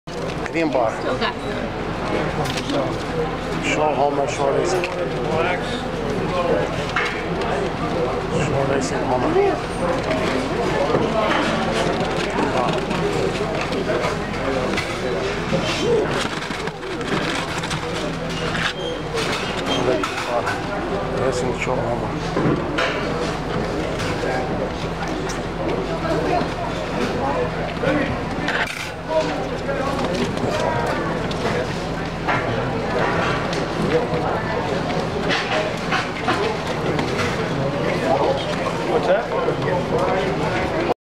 Show Homer cream Bar
Show Homer cream bar, nice bird at 11th Annual Big Apple Invitational Show Old Greenwich Connecticut